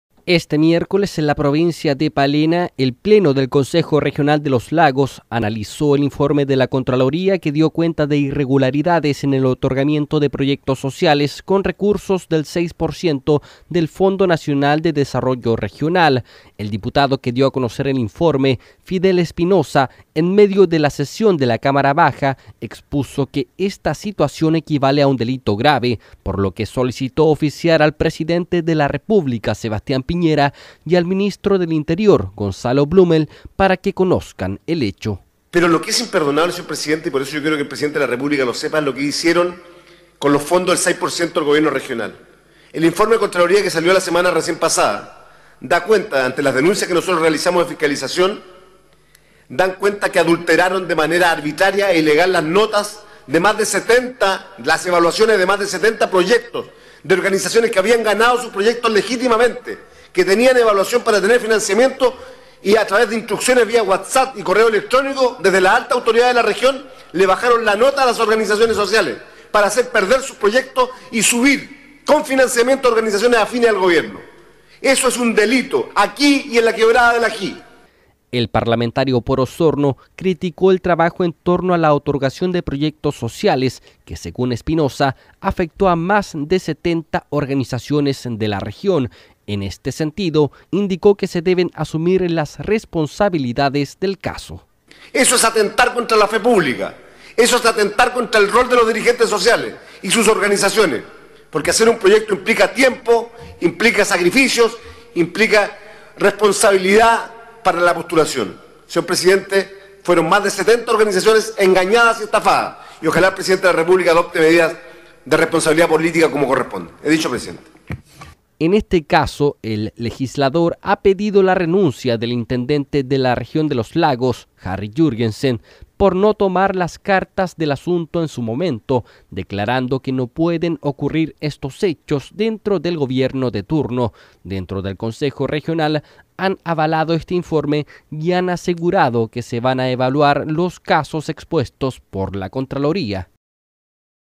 Diputado Espinoza expuso sobre irregularidades en el otorgamiento de proyectos sociales
El diputado Fidel Espinoza expuso en la Cámara Baja el informe de Contraloría que dio a conocer irregularidades en otorgamiento de proyectos sociales en la región. En la oportunidad ofició al presidente Sebastian Piñera para que tome cartas en el asunto.